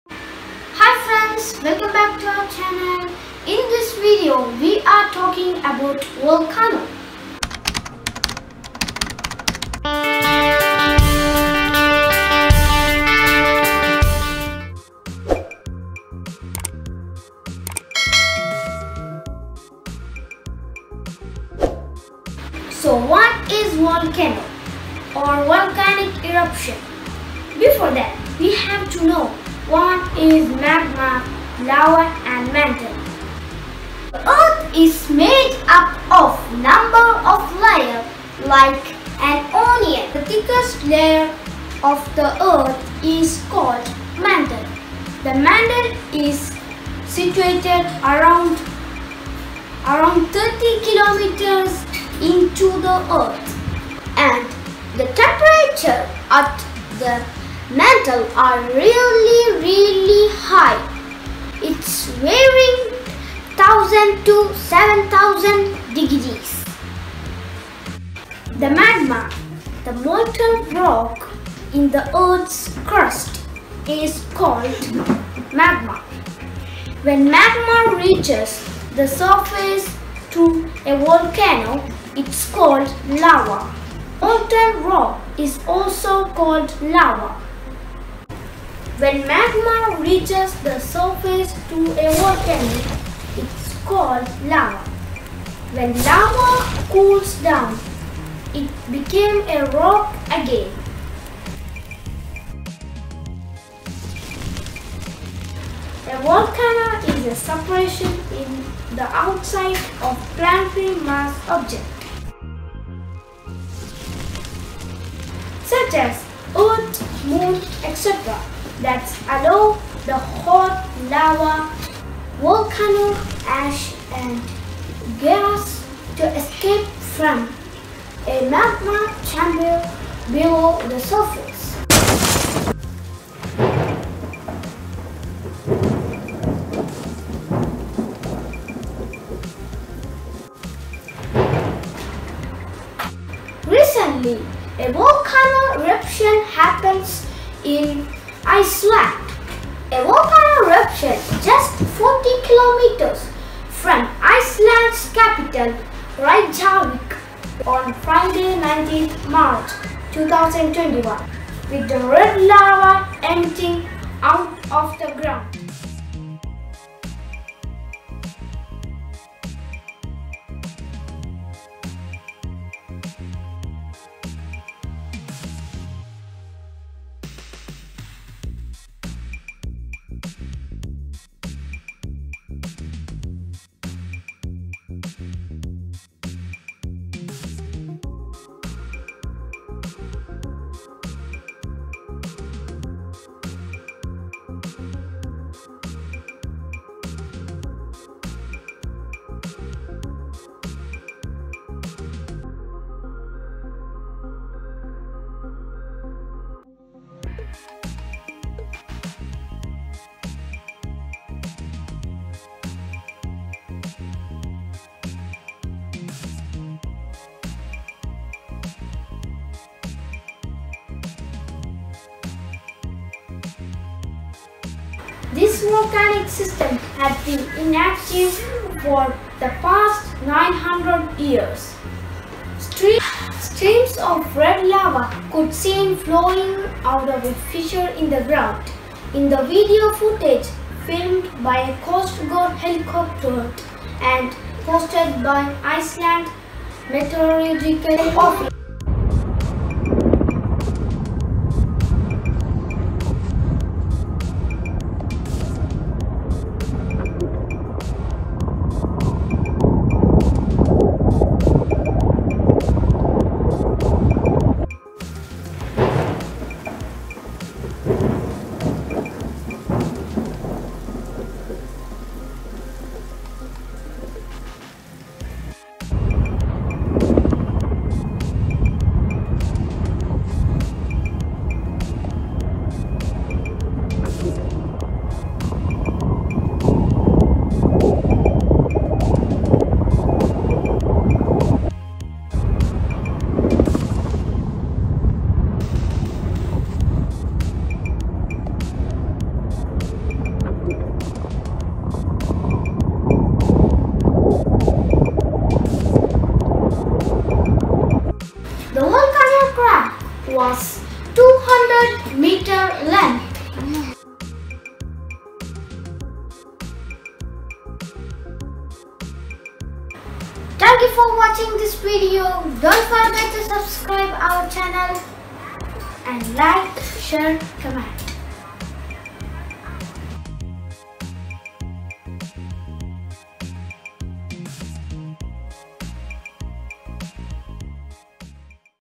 About Volcano Eruption - All About Volcano
Volcano Eruption
This video about Volcano Eruption and a Small briefing about Icelandic Volcano Eruption